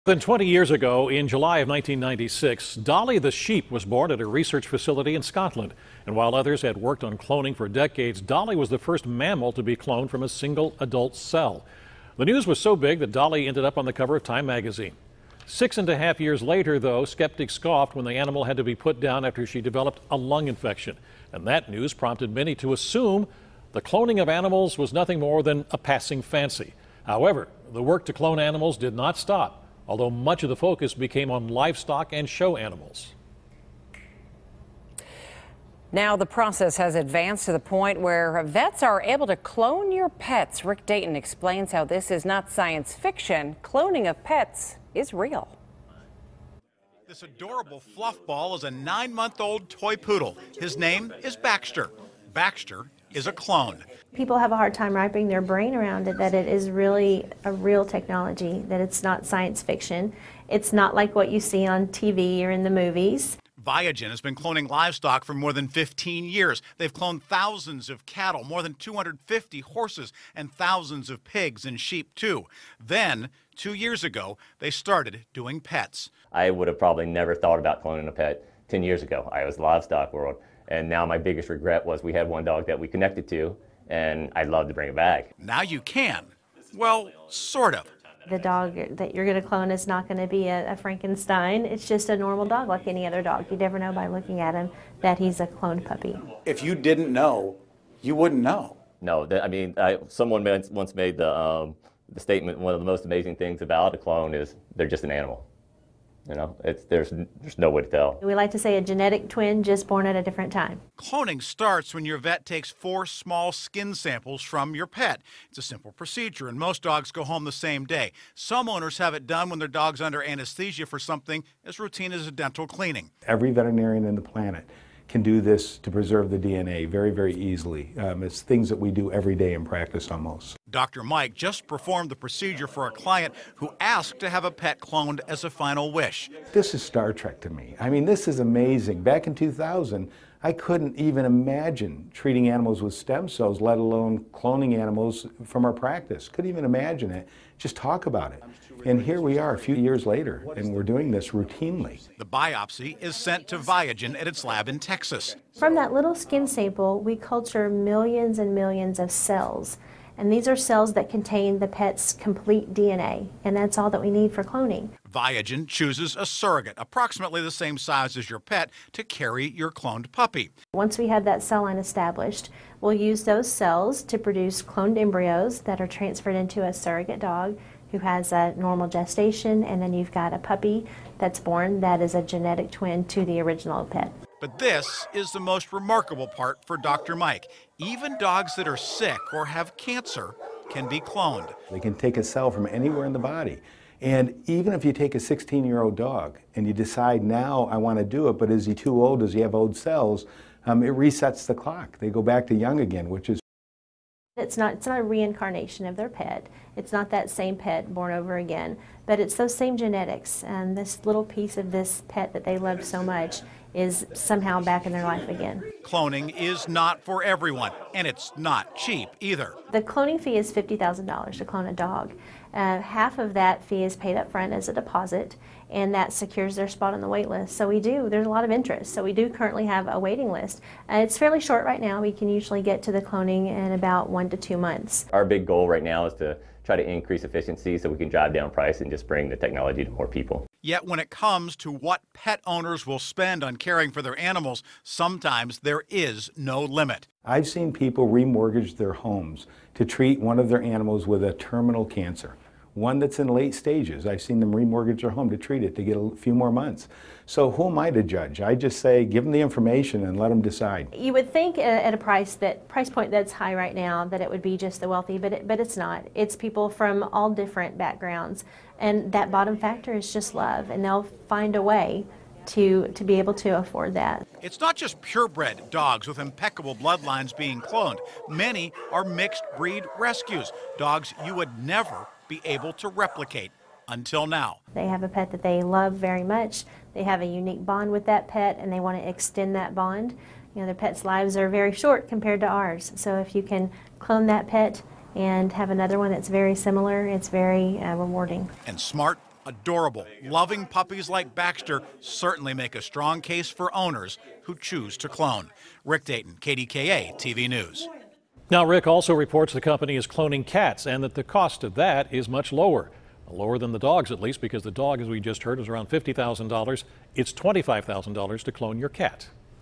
Science Fiction No More: Cloning Now An Option For Pet Owners
The cloning process has advanced to the point where veterinarians and scientists can clone your pets; KDKA's Rick Dayton reports.